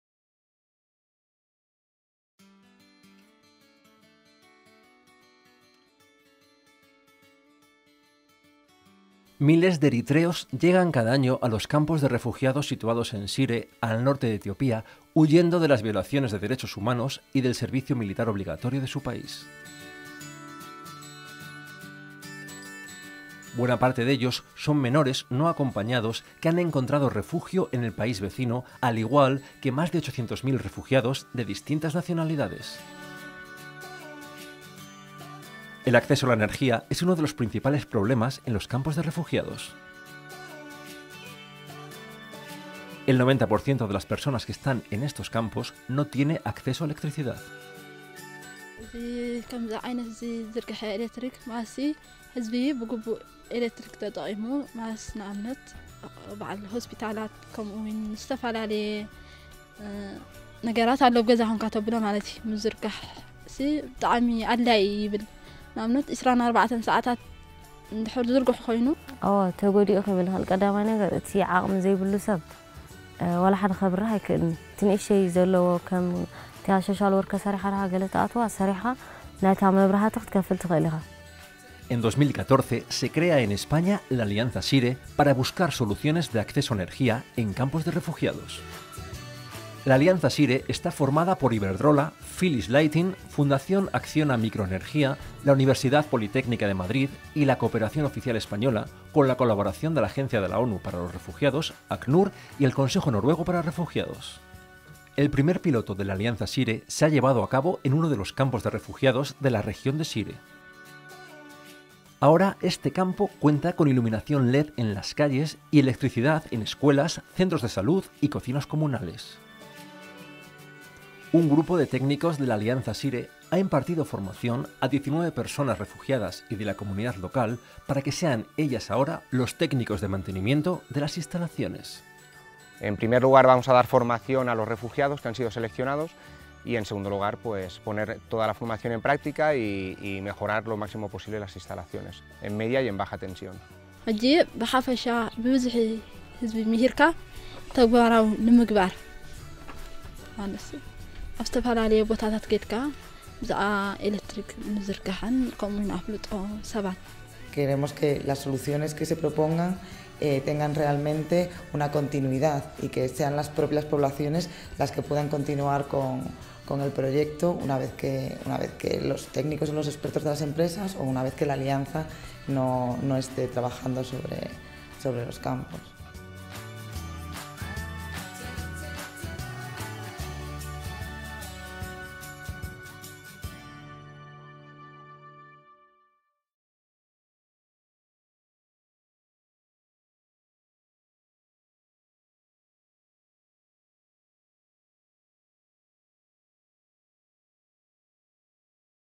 Alianza Shire. Energy access to refugees
Alianza Shire is an innovation platform to develop solutions for improving energy supply services and quality of life in refugee camps, formed by Acciona Microenergía Foundation, The Spanish Agency for International Development Cooperation (AECID), Iberdrola, The Innovation and Technology for Development Centre (Universidad Politécnica de Madrid) and Philips Lighting Spain, with the collaboration of the United Nations High Commissioner for Refugees (UNHCR) and the Norwegian Refugee Council (NRC)

The first pilot project has been developed in Shire, North of Ethiopia